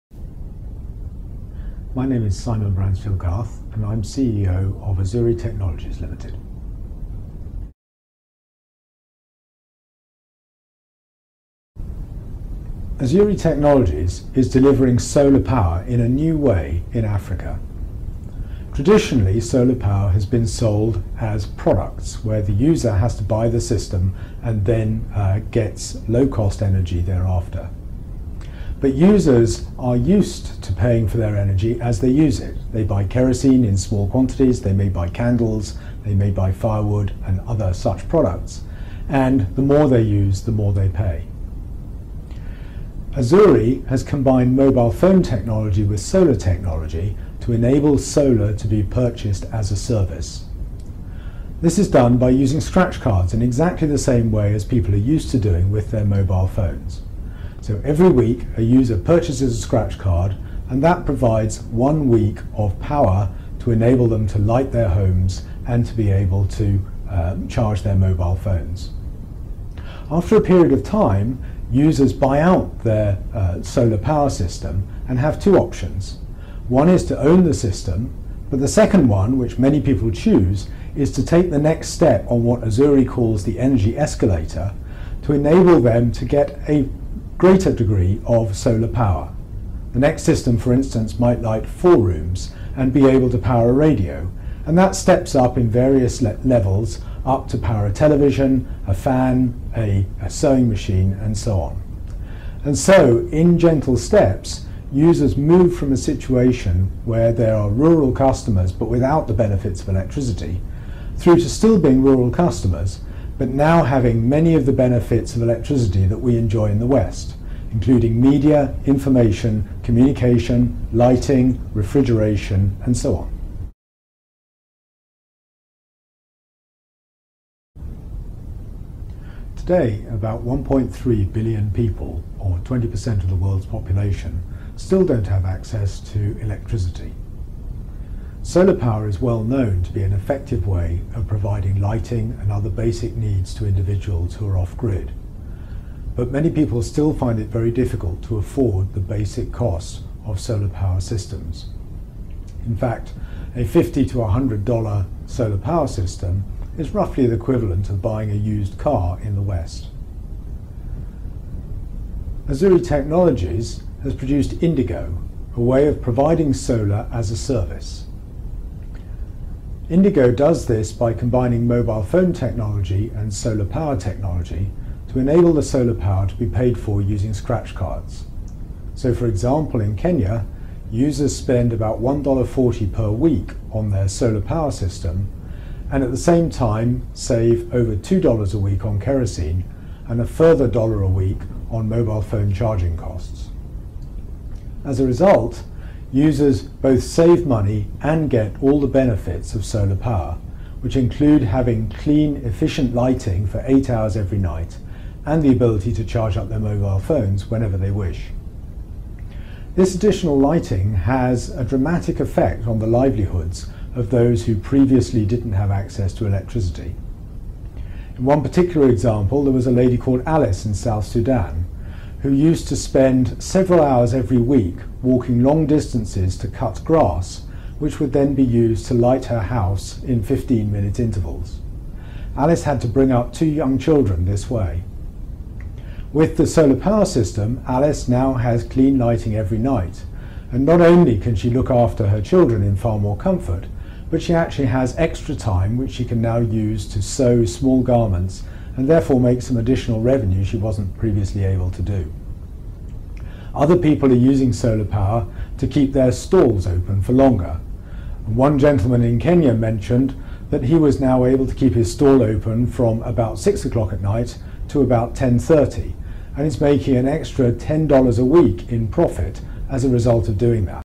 Azuri Technologies named Technology Pioneer 2013 by World Economic Forum
Award recognises innovative approach to tackling energy access. Azuri was selected for its contribution to delivering affordable and clean energy to the world's off-grid population, improving global energy access through its Indigo pay-as-you-go solar solution.